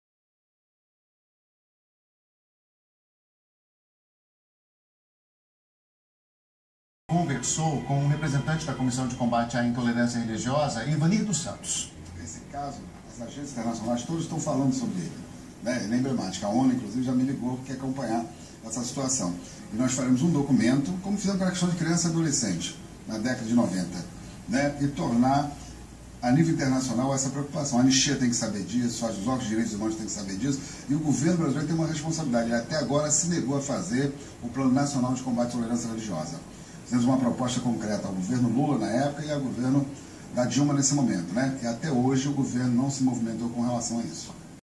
Entrevista com Babalaô Ivanir dos Santos GloboNews
O Babalaô Ivanir dos Santos representante da Comissão de Combate à Intolerância Religiosa do Rio de Janeiro, numa entrevista na GloboNews 19/06/2015.